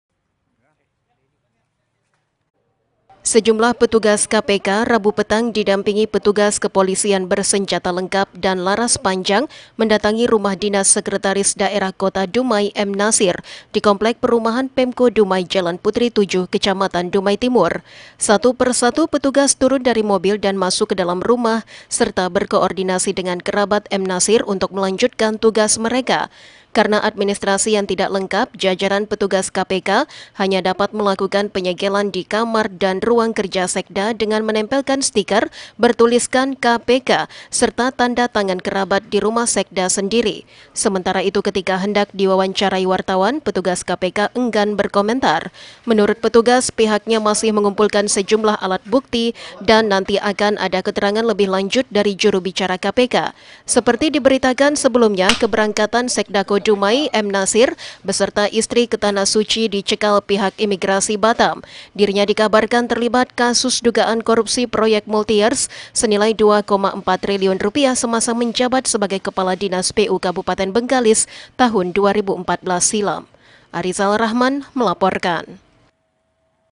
KPK SEGEL KAMAR DAN MEJA KERJA SEKDA DUMAI
Komisi pemberantasan Korupsi, melakukan tahap lanjutan penyidikan, keterkaitan Sekretaris daerah kota Dumai, M Nasir, dalam proyek Multiyeas Bengakalis tahun 2014 silam, dengan mendatangi rumah dinas Sekda, rabu petang. Namun, karena Administrasi tidak lengkap, KPK hanya menyegel kamar dan ruang kerja M Nasir.